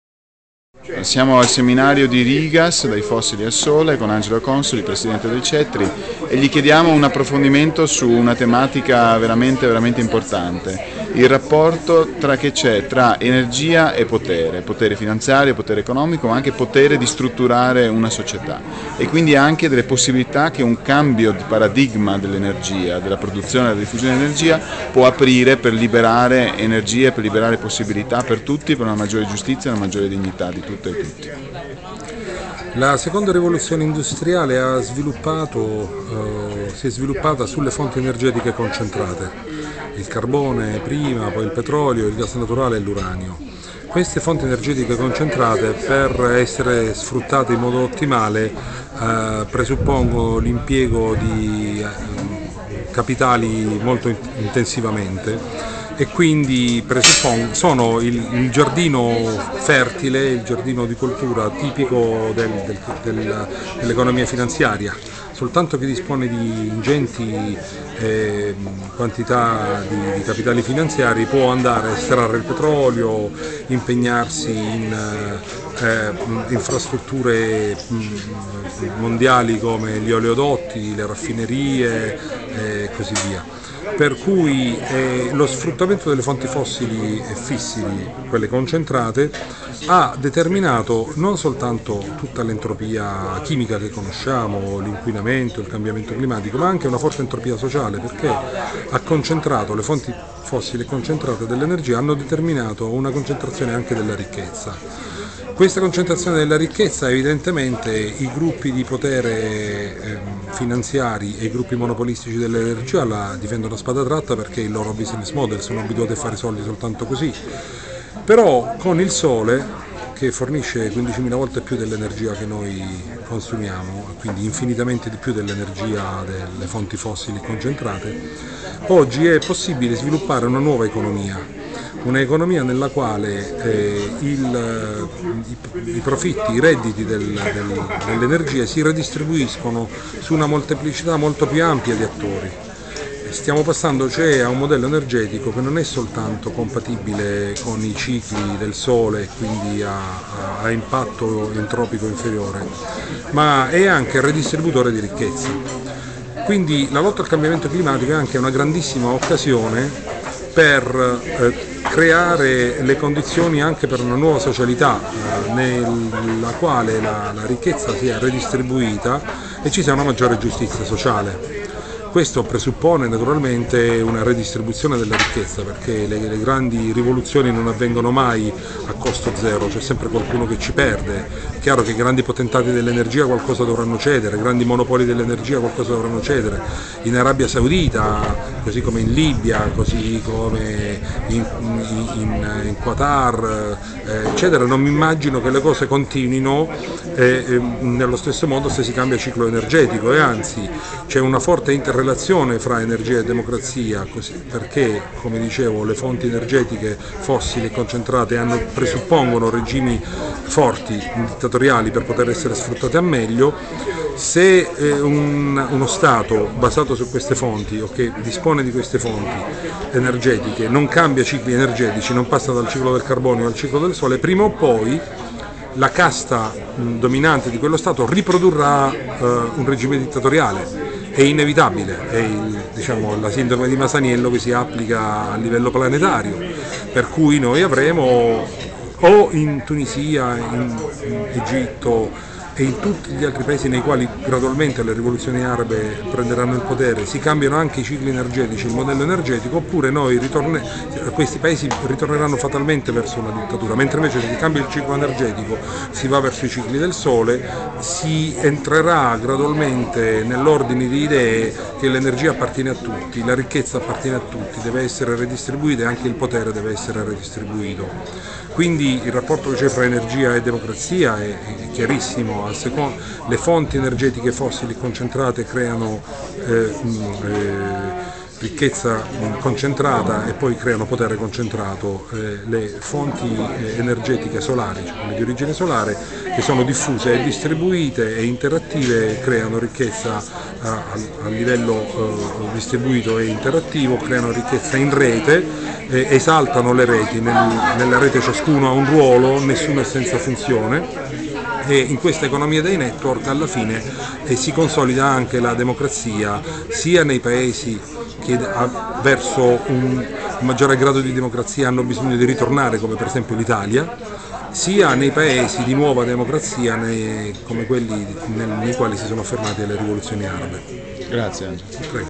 R.I.G.A.S - Intervista a Marco Consoli
Giornata di studio "Dal fossile al sole" Roma 16 settembre 2011